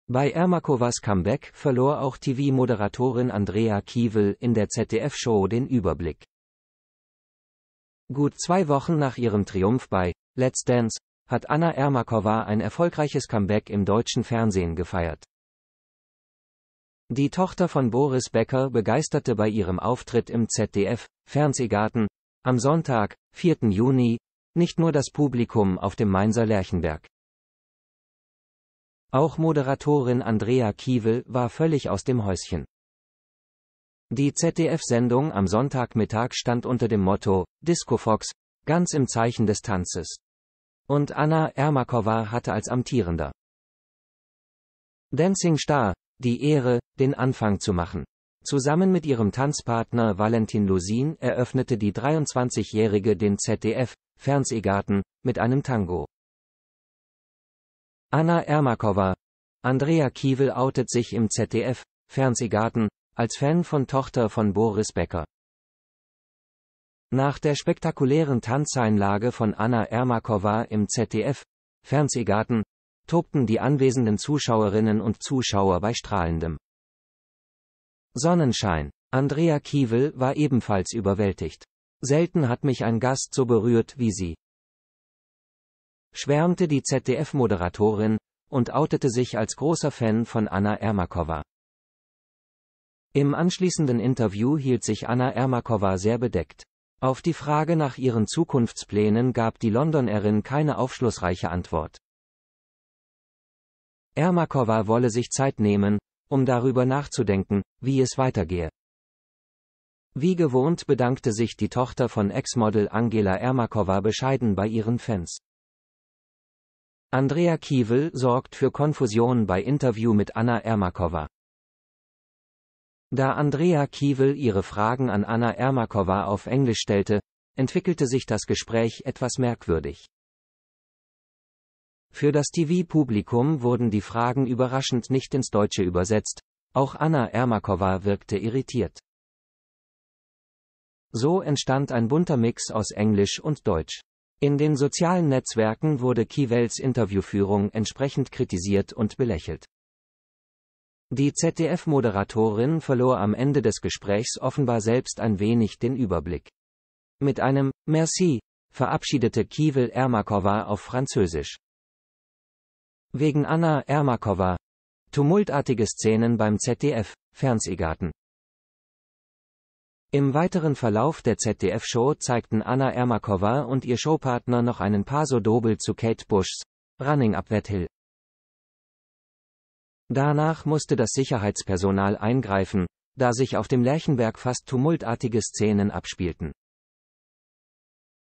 Andrea Kiewel verspottet Anna Ermakova! Das ist Verachtung!
Bei Ermakovas Comeback verlor auch TV-Moderatorin Andrea Kiewel in der ZDF-Show den Überblick.